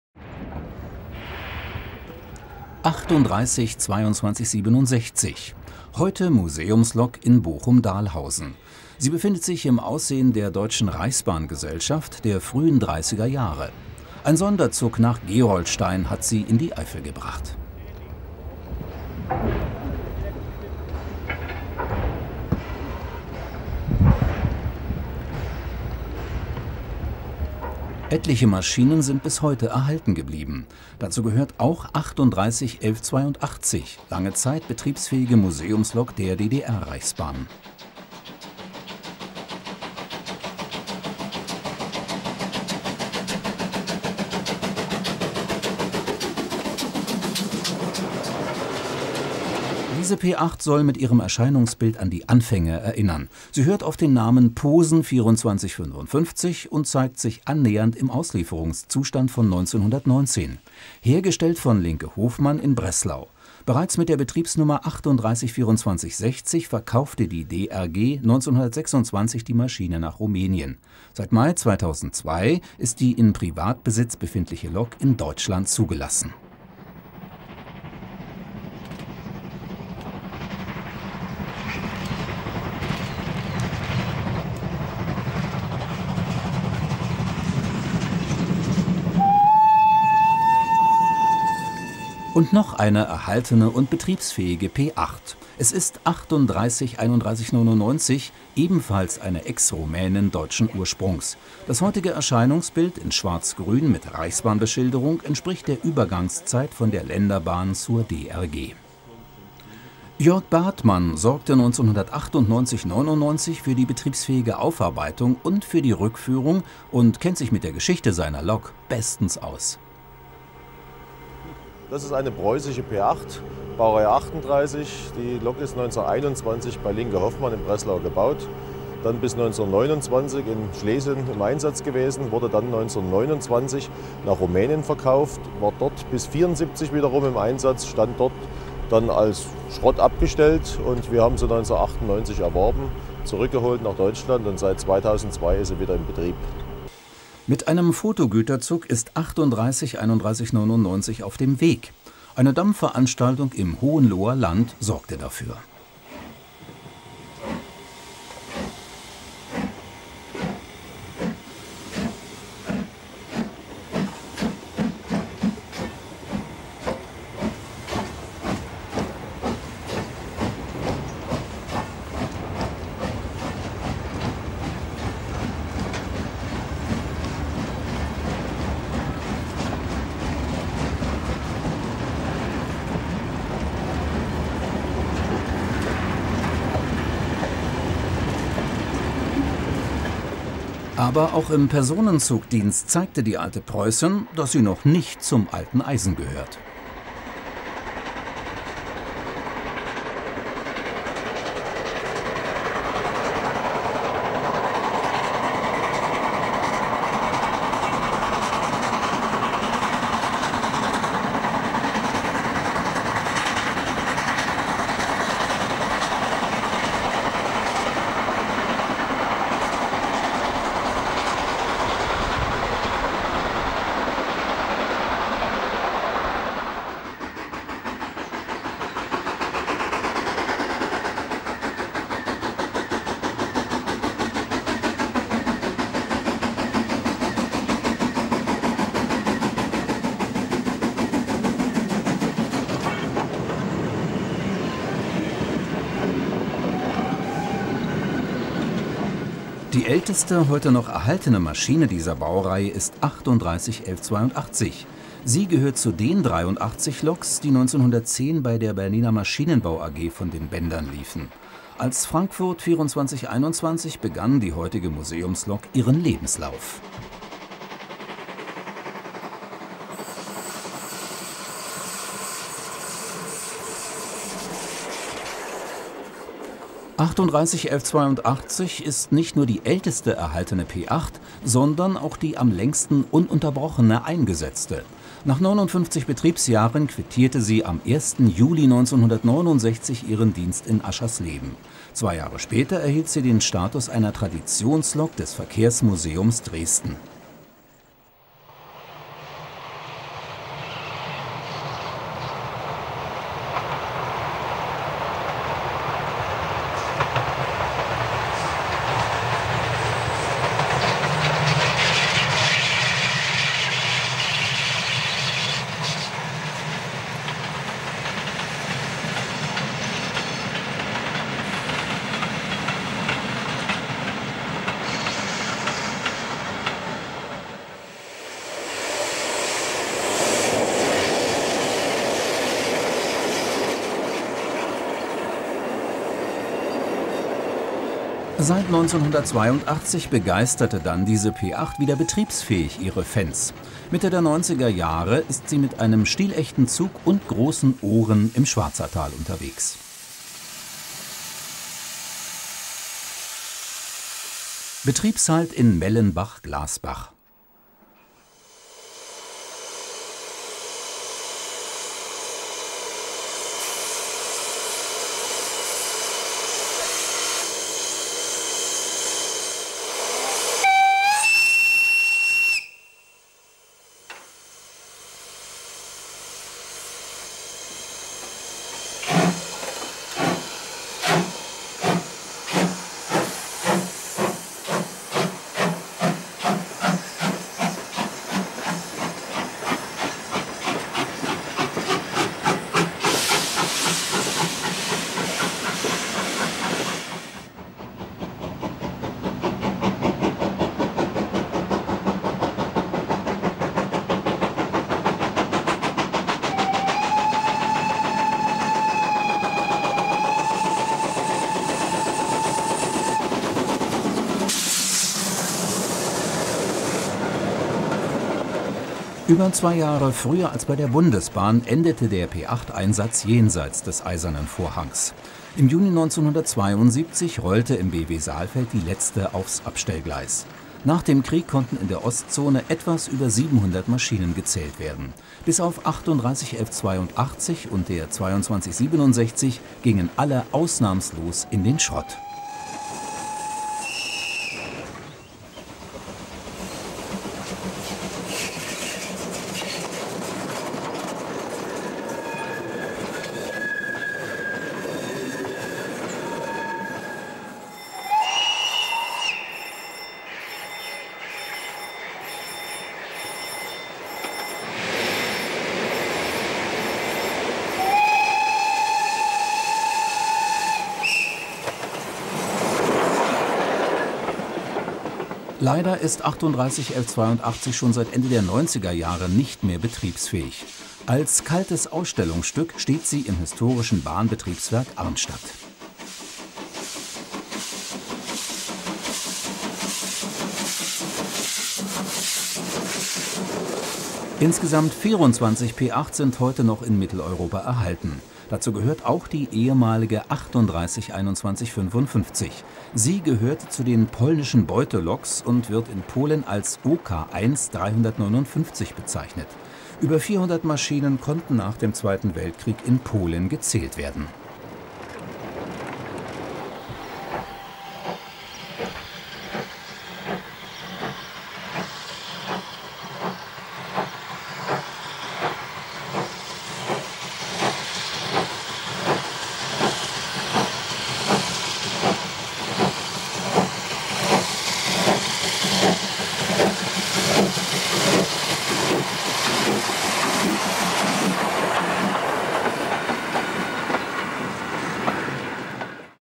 Mädchen für alles -- preußische P8
Im Jahre 1906 kommt die erste P8 auf Gleise. Es ist einer der ersten Heißdampfloks. Fast 4000 Stück werden gebaut. Einige davon sind erhalten geblieben, im Museum ausgestellt oder aktiv im Sonderzugdienst unterwegs. In unserem kurzen Film zeigen wir Ihnen fünf aktive P8 im Sonderzugdienst oder bei Plandampfveranstaltungen. Viel Spaß beim Anschauen. Hier mehr zur P8